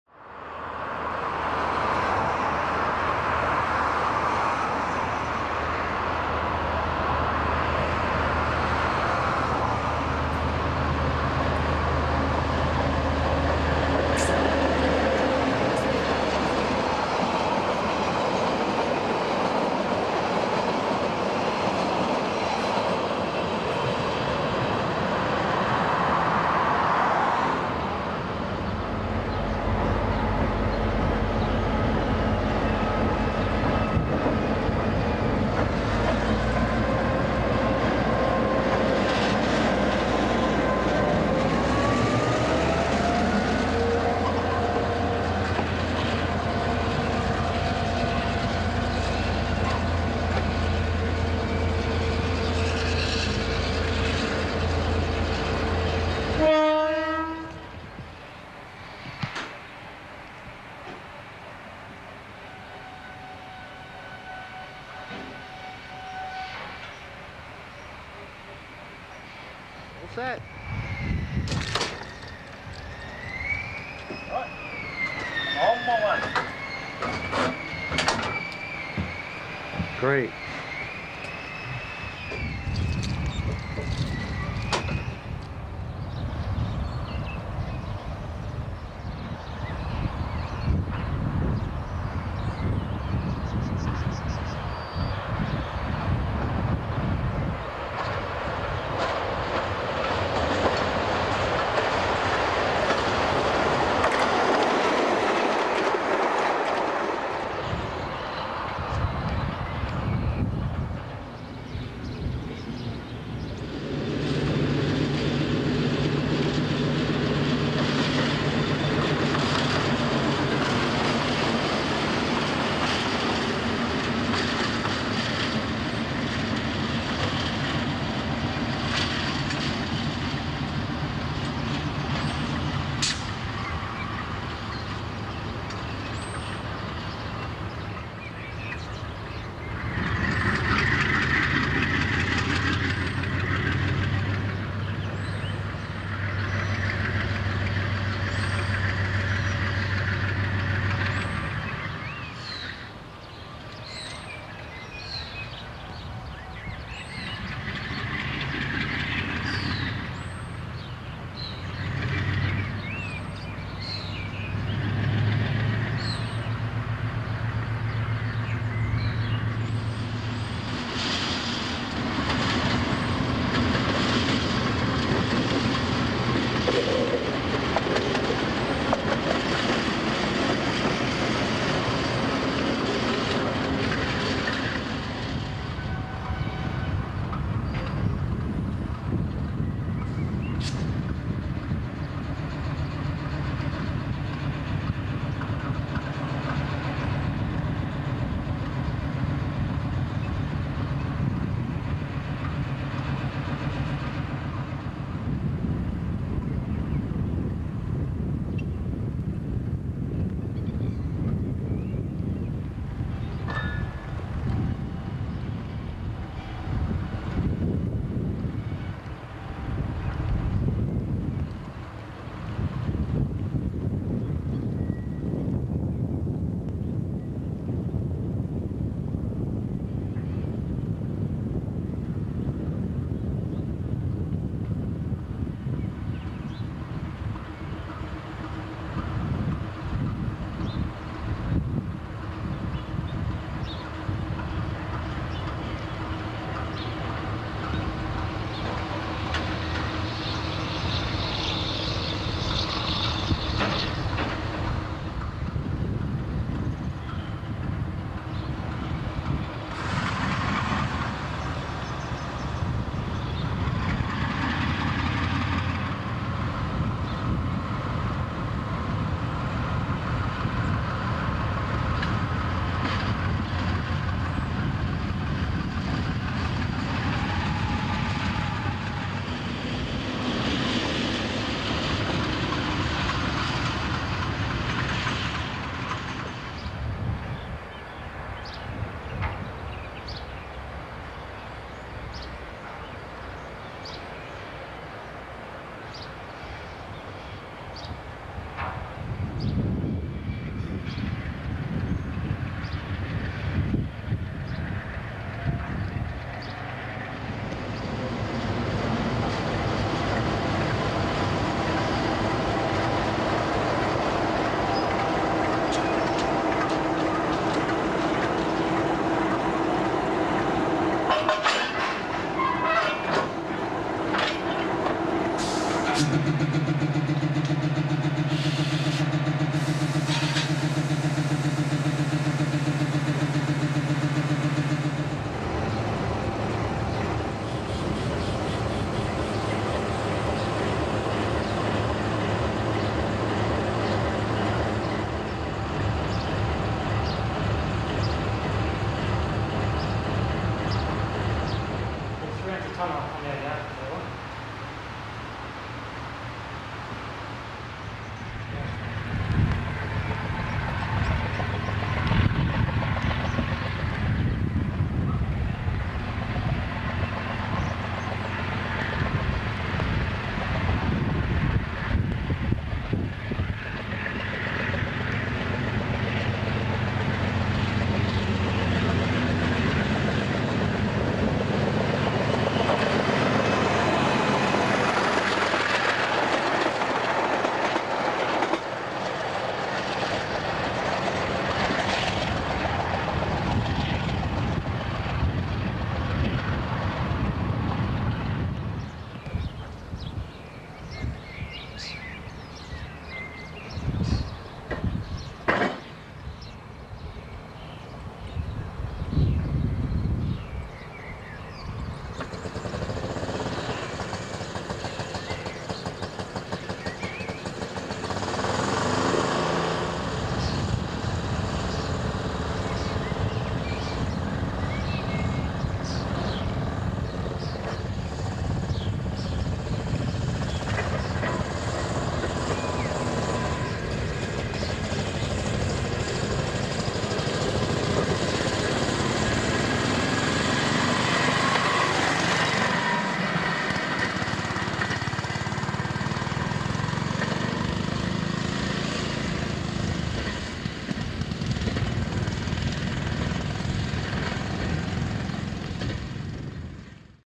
New Zealand Masterton
A visit to Masterton on the North Island of New Zealand.  This was the last station on the NZR system to use the Electric Token system for train movements on single line, and it had just closed prior to my visit.  The station utility man was kind enough to let me view the block instruments and operated the "somersaulting" type semaphore for the camera.  He also let me into the shed where the small shunting loco was kept.  This station was the end of a regional commuter run from Wellington, and we witnessed the big GE being turned by hand on the turntable.